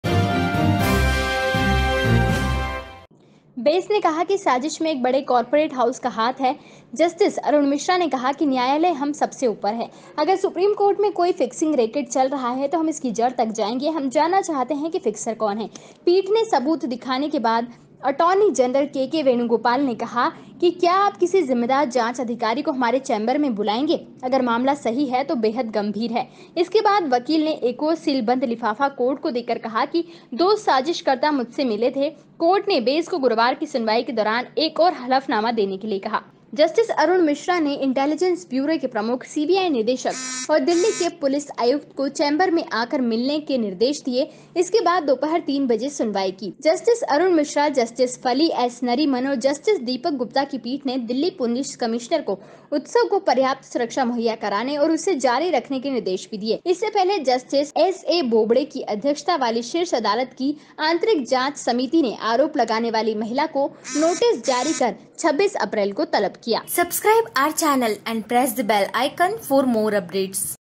सुप्रीम कोर्ट के फिक्सिंग रैकेट को बेनकाब करेंगे
मामला चीफ जस्टिस पर  आरोप का |